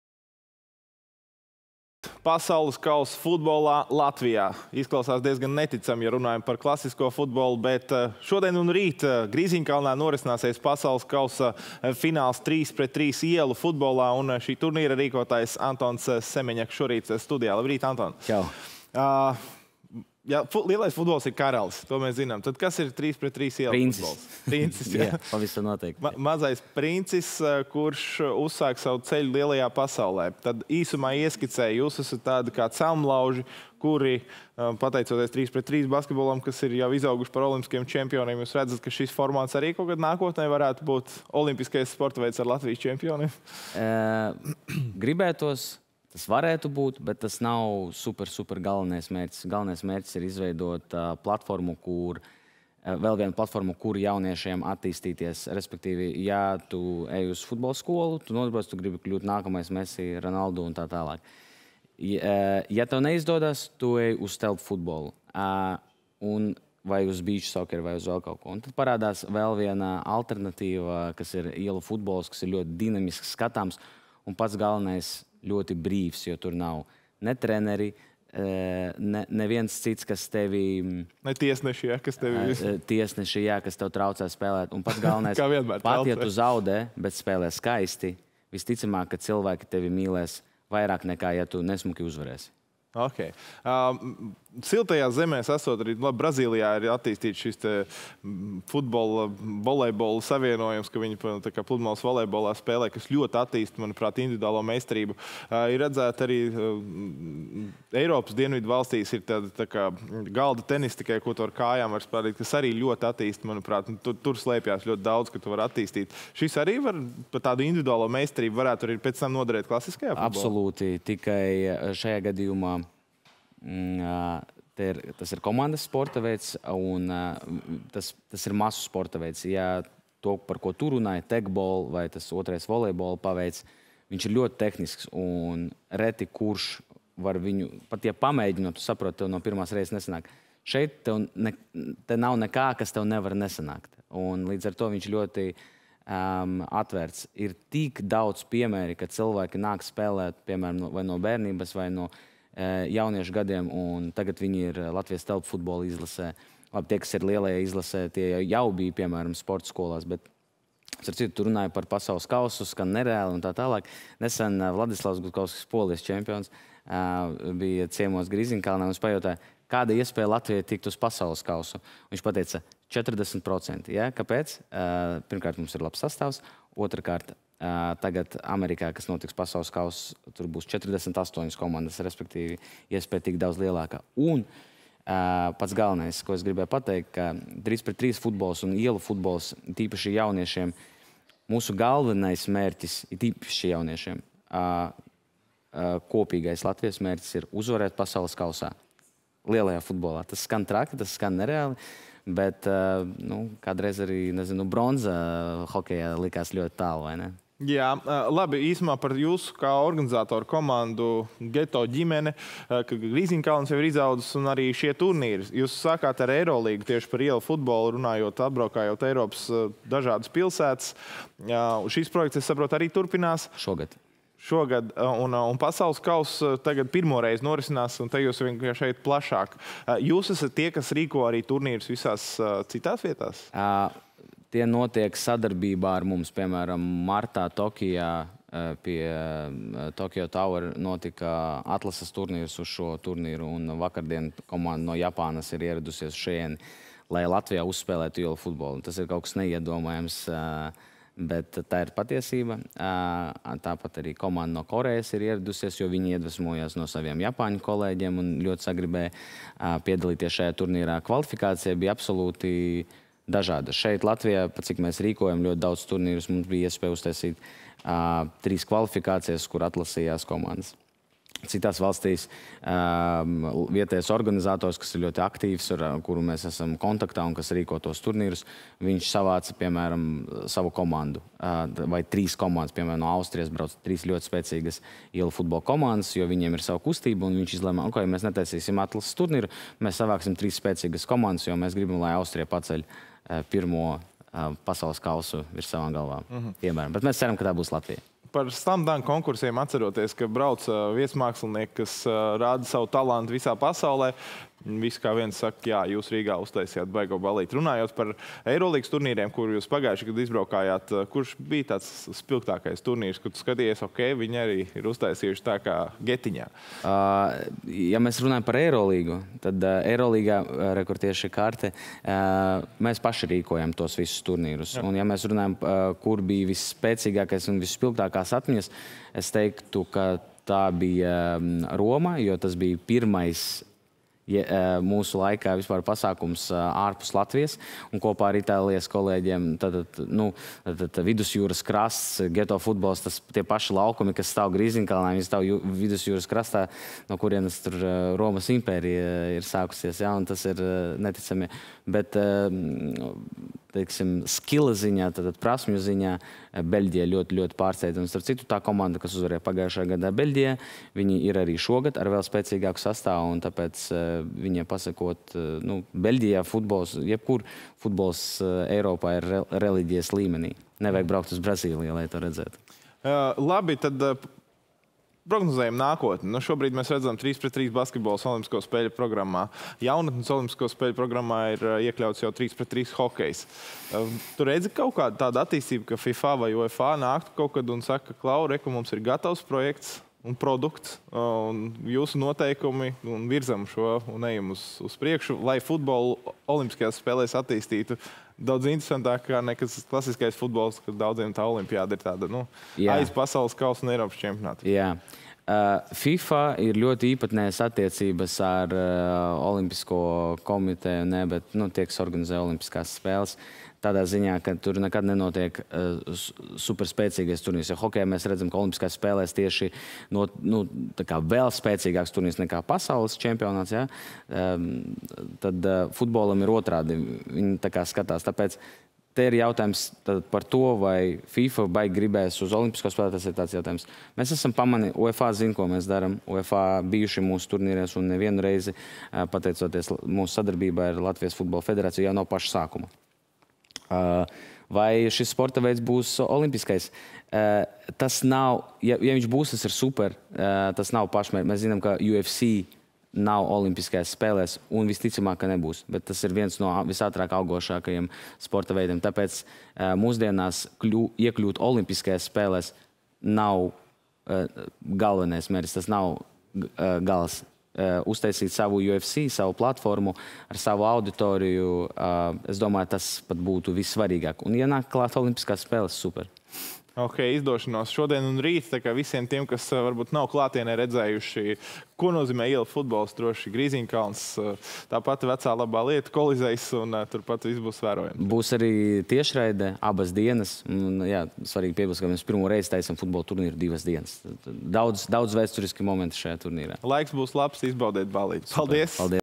Pasaules kauss 3×3 ielu futbolā
LTV sporta ziņu redaktora Reiā Ošenieka saruna ar Pasaules kausa ielu futbolā rīkotāju Antonu Semeņaku